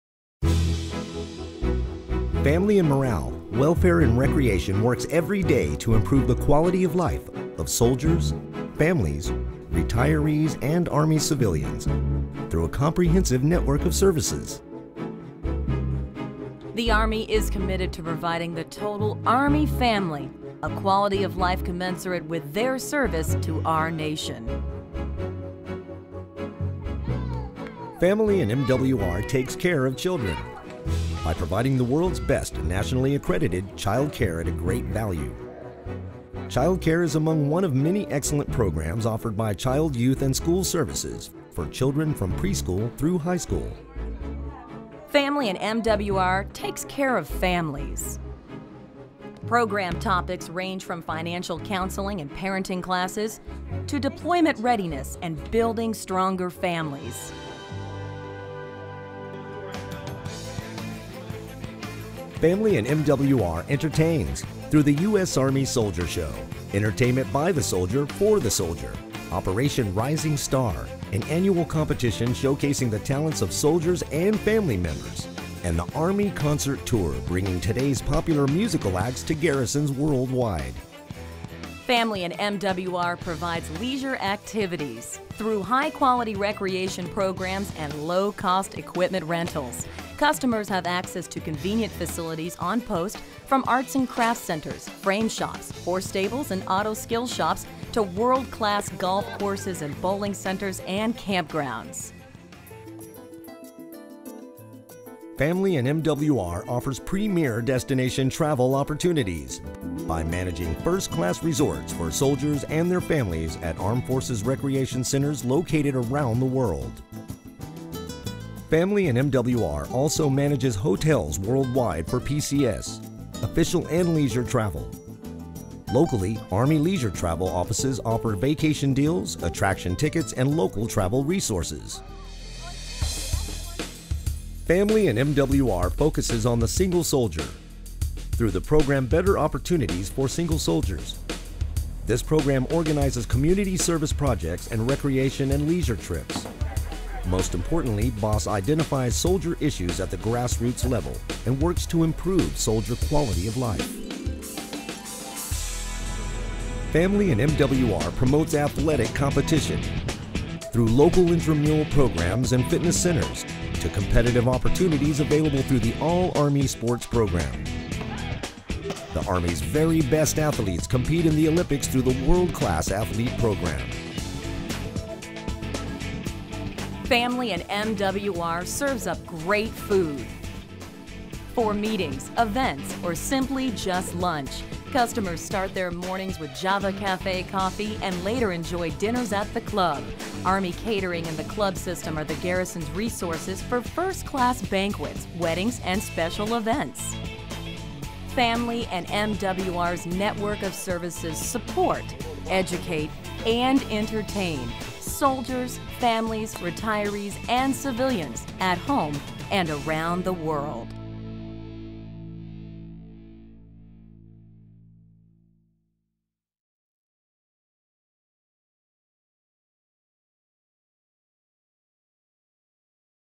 Family and MWR A Network of Services
Family and MWR is a comprehensive network of quality support and leisure services that enhances the lives of soldiers, civilians, families, military retirees and other eligible participants. Watch this video to learn more about Family and MWR.